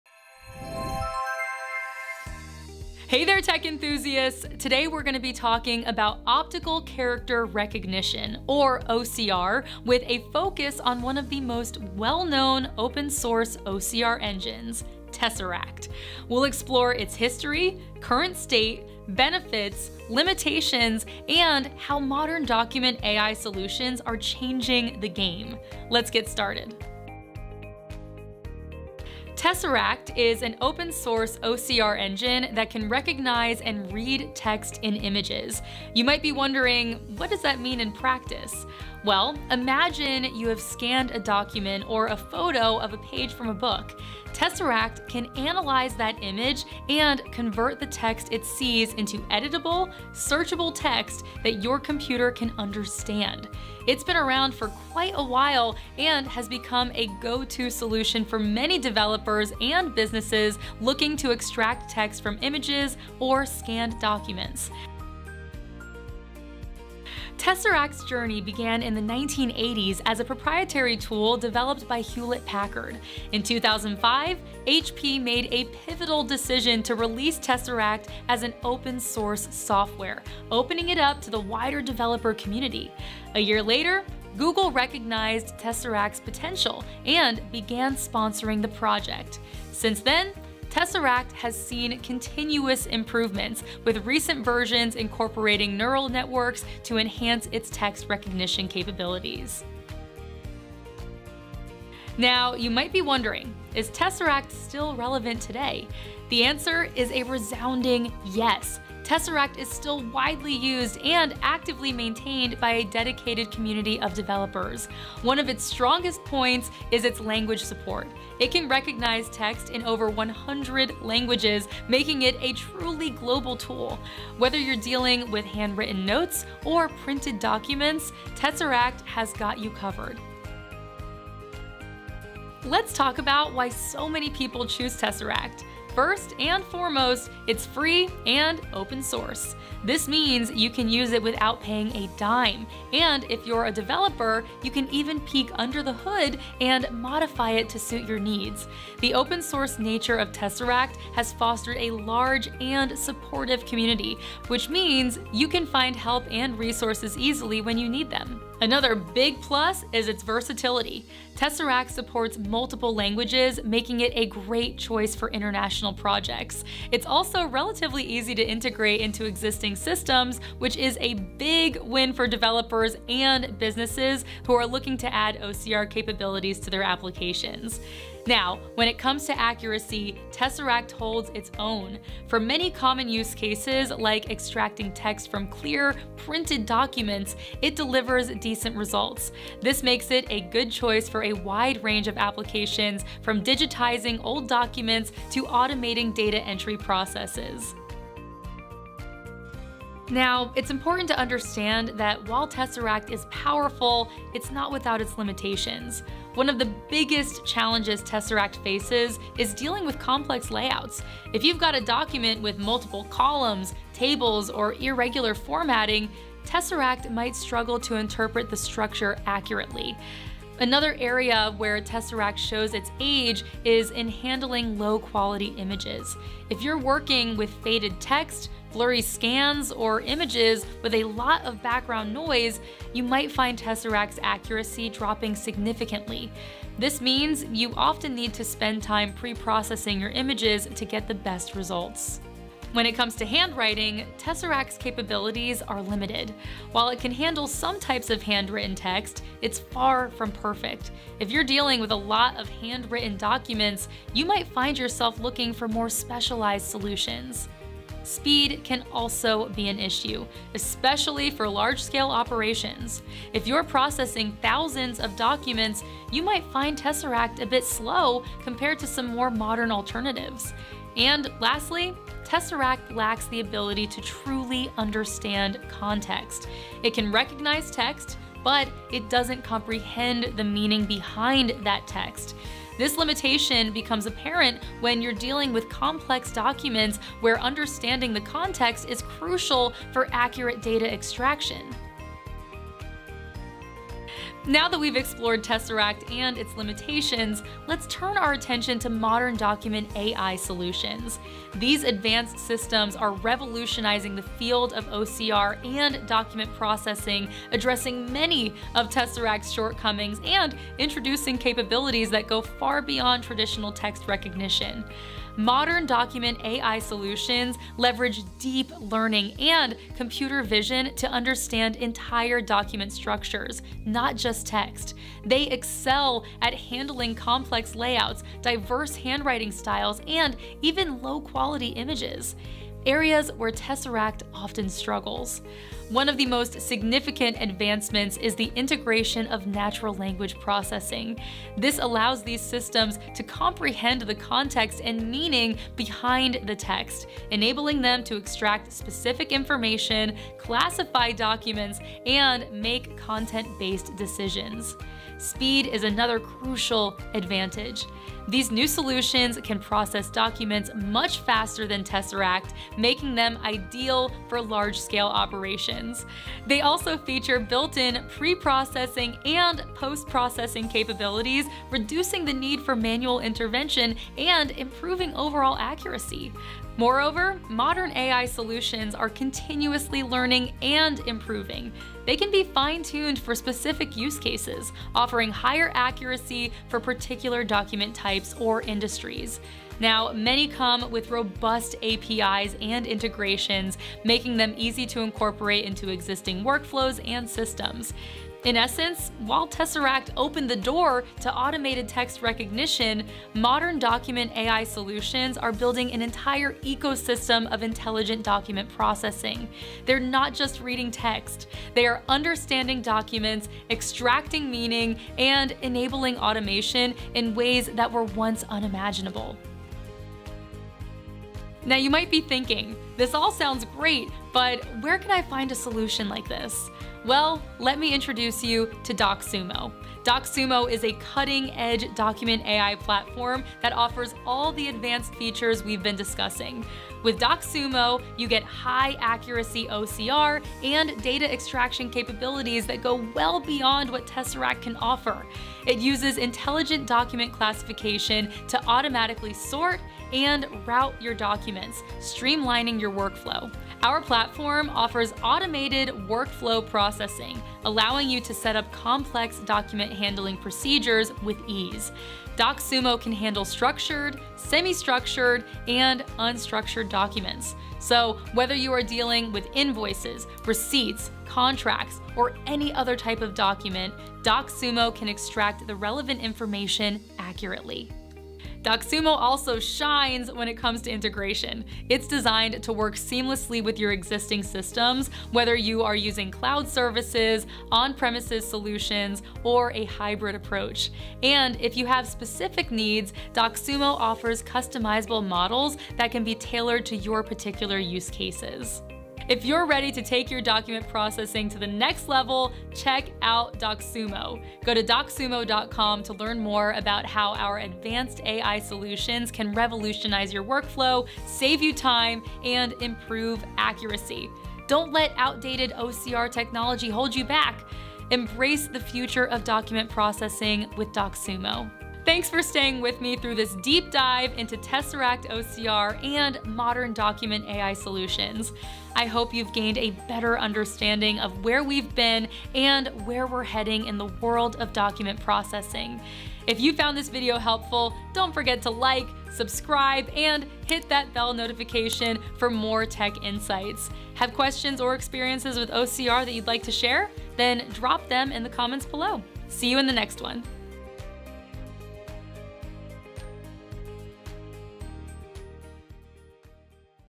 Tesseract OCR: What is it and is it the BEST for you?
Discover the power of Tesseract OCR! This video breaks down what Tesseract optical character recognition is, its history, and how it compares to modern AI solutions. Ready to revolutionize your document processing?

🕒 Chapters:
00:00 What is Tesseract-OCR?
01:05 History of Tesseract-OCR
01:40 Tesseract OCR's current situation
02:11 Benefits of Tesseract OCR
03:18 Limitations of Tesseract OCR
04:53 The Rise of Modern Document AI Solutions
07:00 Docsumo: A Powerful Alternative to Tesseract OCR

Dive deep into the world of Tesseract OCR and discover how this open-source optical character recognition engine has transformed document processing. From its humble beginnings to its current capabilities, we explore the ins and outs of Tesseract-OCR and compare it with cutting-edge AI-powered alternatives.

Ready to take your document processing to the next level? and experience the future of intelligent document processing today!